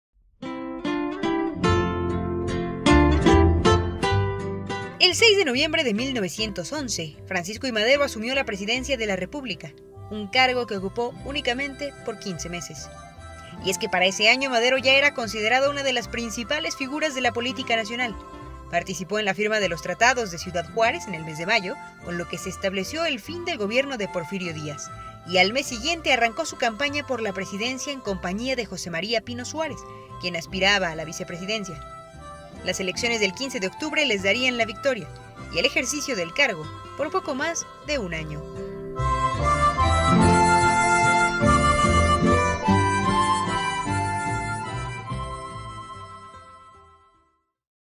Cápsula: Francisco I. Madero, presidente de México
Oro Noticias Puebla te da parte de la historia de uno de los presidentes más importantes de México: Francisco I. Madero.